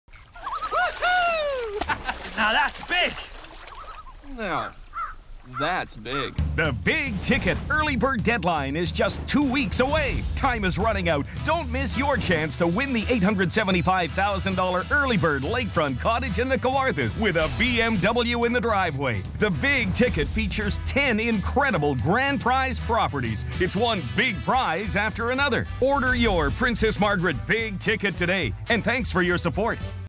"Fishing"
A spot done for the PMCC Home Lottery Fund. With less than two weeks remaining to the Early Bird Deadline for the 2013 Princess Margaret Cancer Centre Home Lottery, time is running out! Don't miss out on your chance to win a beautiful $875,000 cottage in the Kawarthas!